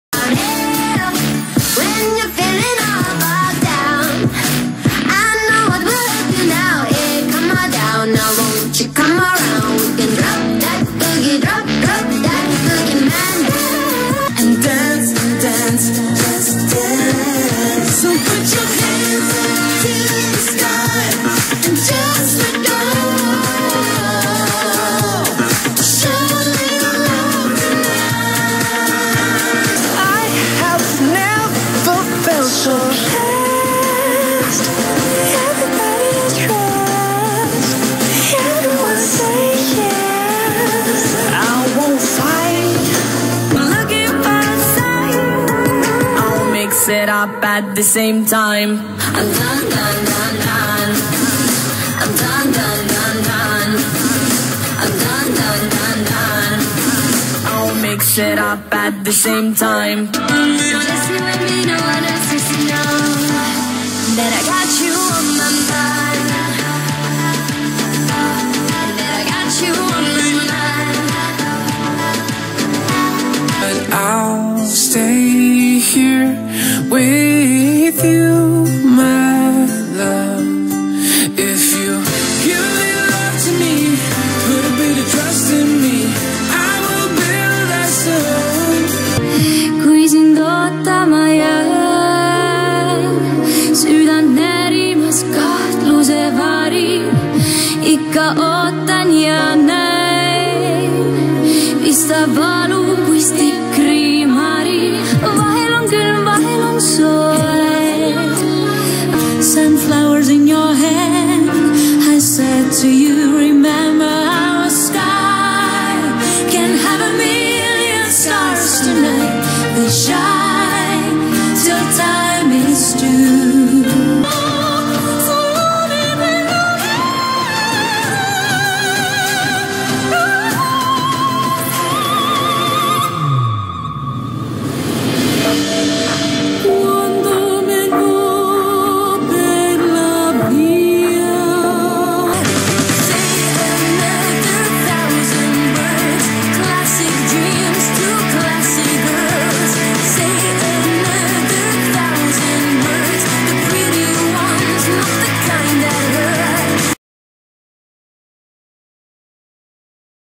Eesti Laul 2018 - My Top 10 - Estonia Eurovision - Song Preview
My top 10 for Eesti Laul 2018 based on the audio preview.
The top 5 and #1 were easy. Only sorting between 2 and 5 was difficult. Enjoy!